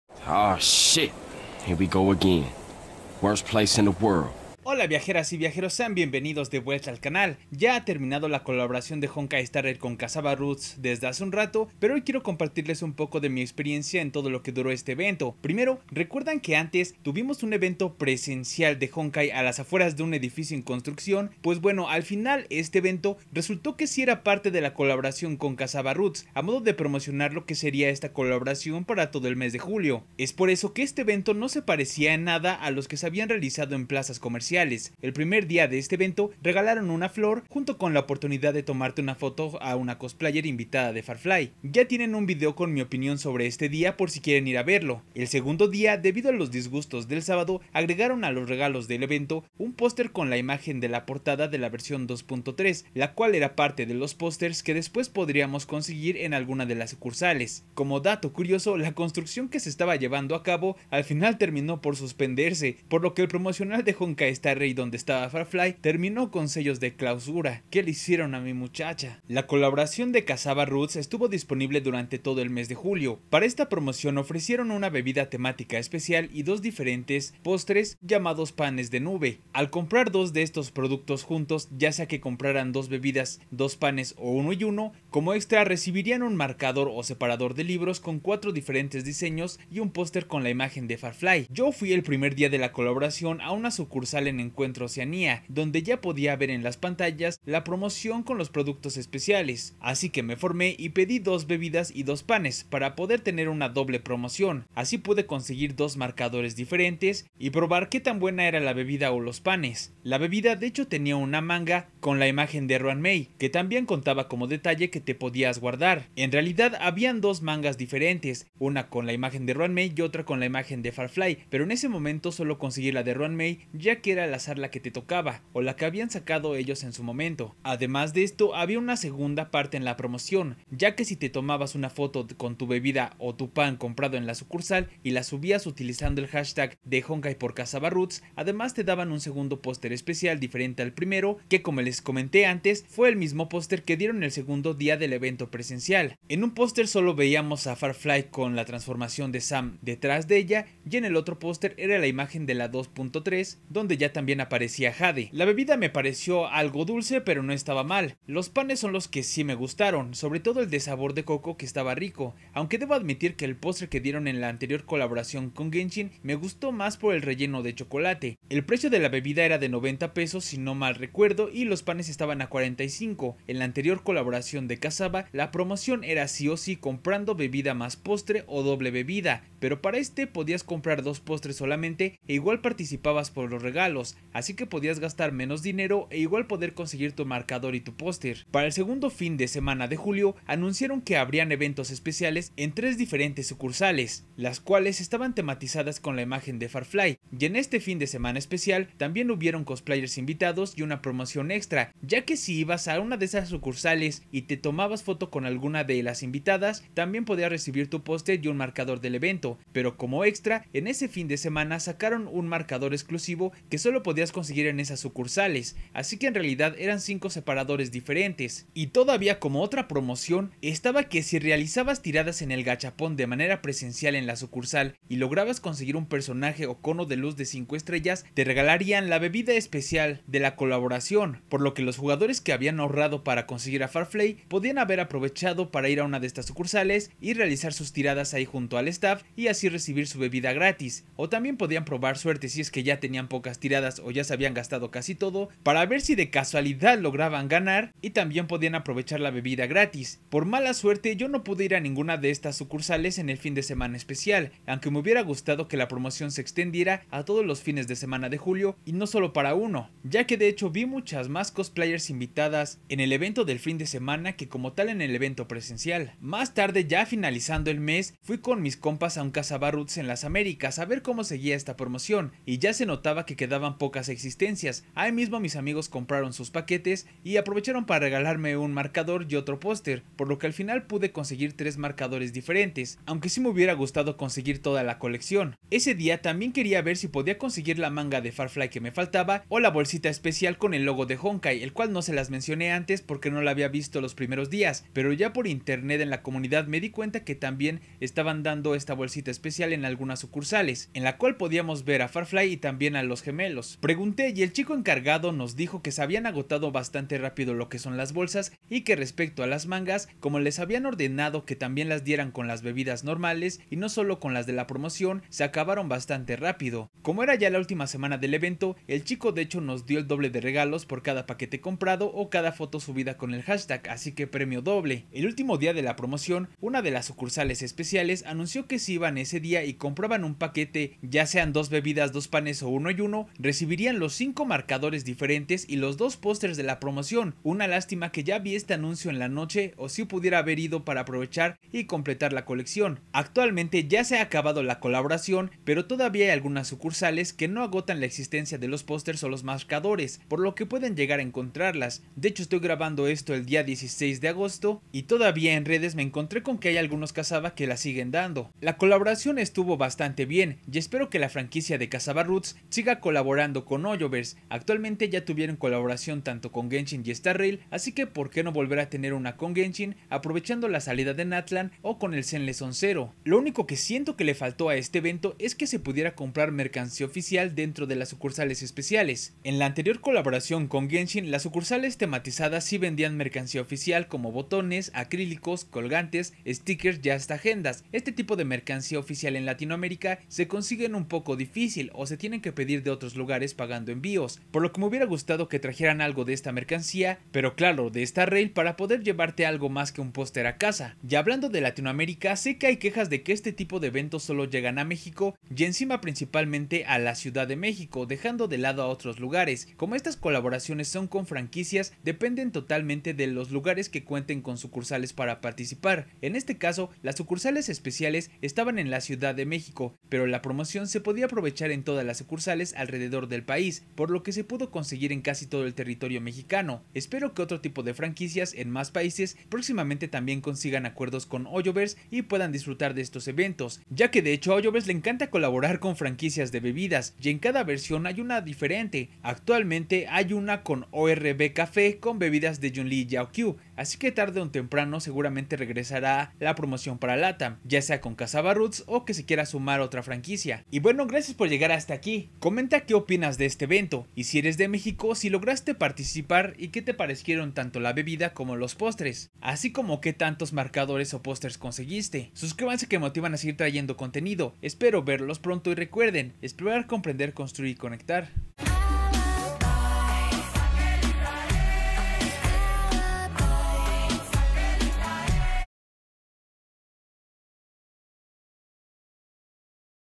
HONKAI: STAR RAIL - ASI FUE LA COLABORACION DE HSRxCASSAVA
Hola viajeras y viajeros tarde un poco pero quiero contarles que tal estuvo el evento de honkai star rail con cassava roots, que me parecio y que consegui de la promocion.

00:00 EVENTO PRESENCIAL
01:09 COLAB X CASSAVA
03:08 EVENTO ESPECIAL
04:26 ULTIMOS DIAS
05:59 OPINION

Si el discord no funciona revisa un video mas reciente